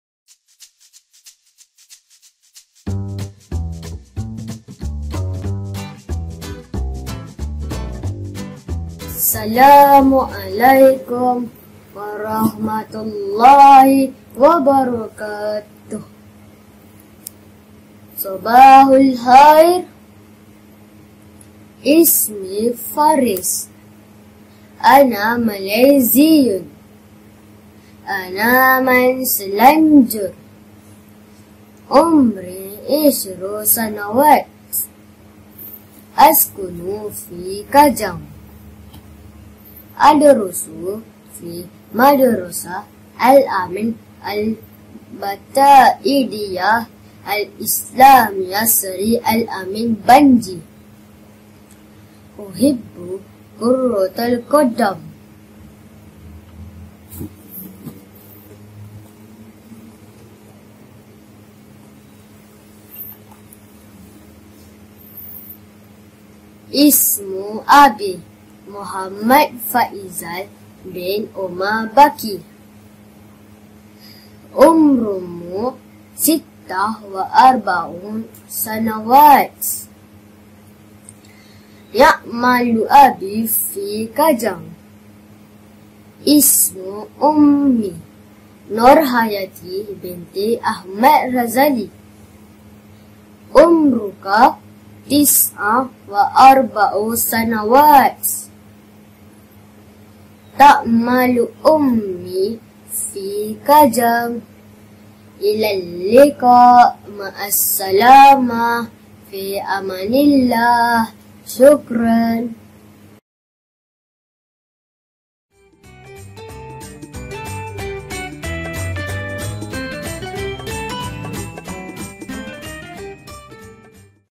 Faris | BahasaArab | Pengenalan Diri
*"Pertandingan Video Taaruf"*🎥📹📽️

Assalamualaikum

Bersempena dengan minggu bahasa arab pada tahun ini, panitia Bahasa Arab akan mengadakan pertandingan video taaruf.

🎉 Penyertaan WAJIB kepada semua murid.
🎉 Pertandingan dilaksanakan pada peringkat kelas sahaja.
🎉 Teks perbualan/taaruf akan diedarkan kepada semua murid melalui guru subjek masing-masing.
🎉 Video mestilah dihantar kepada guru subjek bahasa arab masing-masing melalui applikasi telegram/whatsapp
🎉 Tarikh akhir penghantaran video ada selewat-lewatnya pada hari Selasa (25 Ogos 2020)